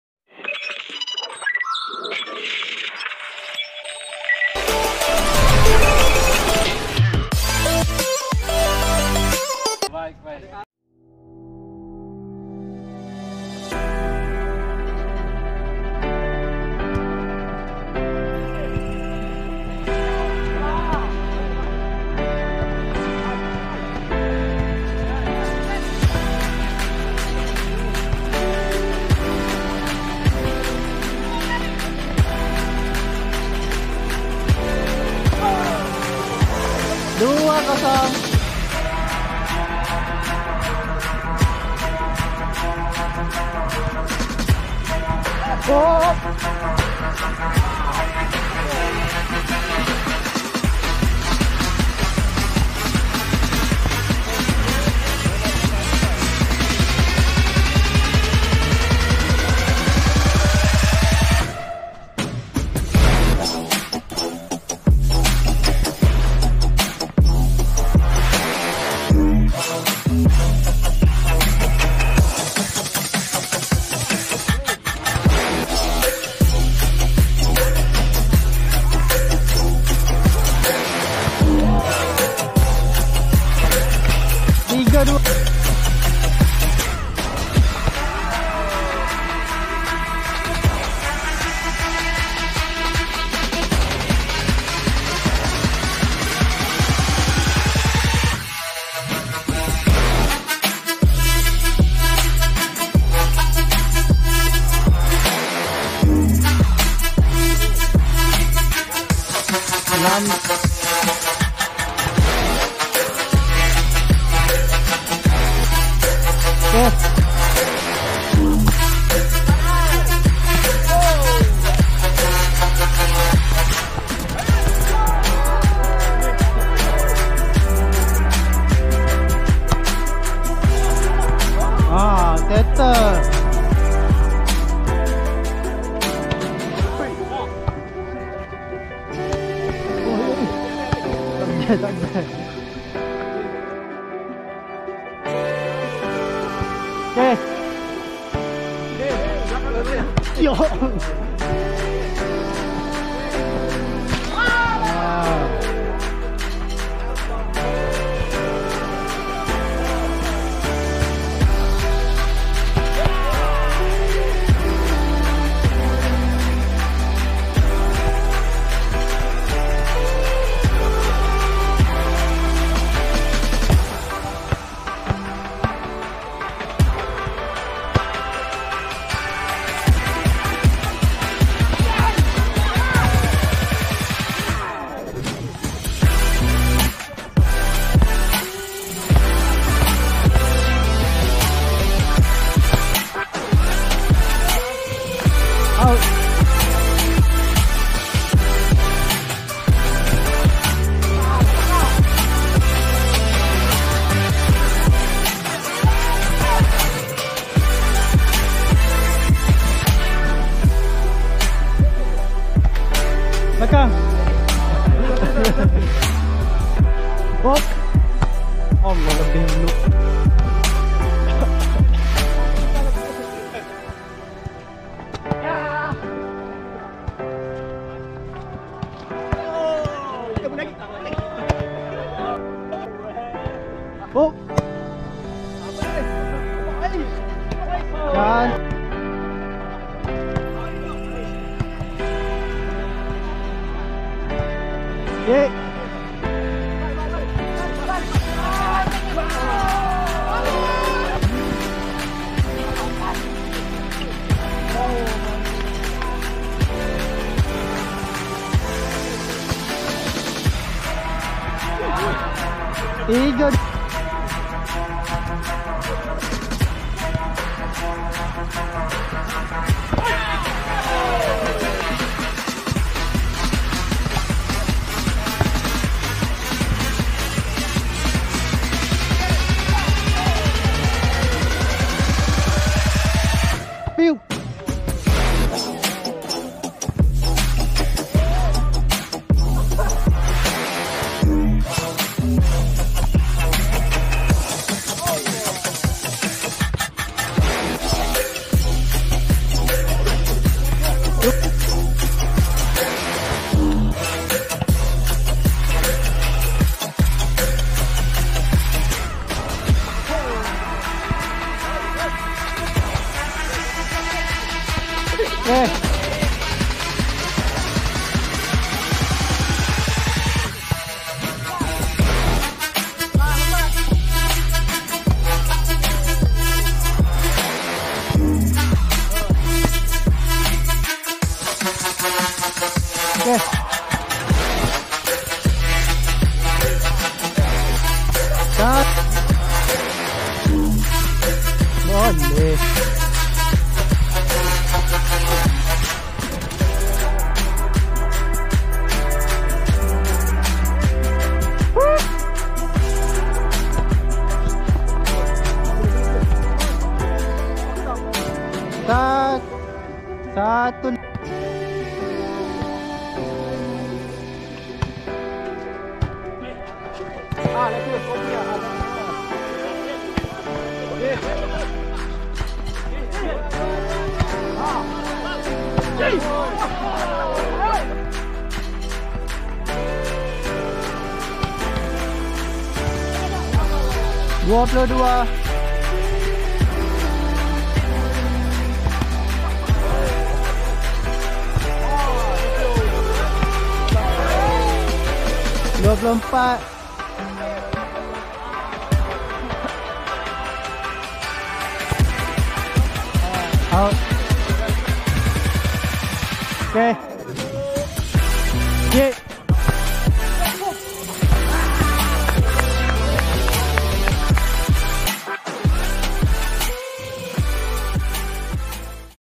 VB 1 VOLLEYBALL FIRST PERSON | BOLA TAMPAR | match 1 set 1| Geng Pensyarah Kolej Vokasional Beaufort
Penyarah Kolej Vokasional Beaufort

Video software edit:  GoPro

Video by: GoPro Max 360